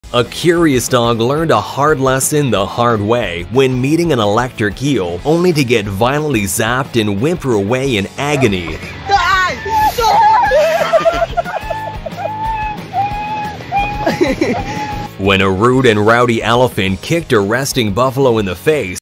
"Incredible Eel's Genius Defense: Outsmarting Dog with Biggest IQ and Intelligence"
"Prepare to be amazed as you witness the incredible defense tactics of an eel with the biggest IQ and intelligence, outsmarting a dog in a remarkable encounter. This extraordinary display of intellect showcases the eel's ability to harness its intelligence to protect itself from a formidable opponent. Watch in awe as the eel employs ingenious strategies to evade and outwit the dog, proving that intelligence knows no bounds in the animal kingdom."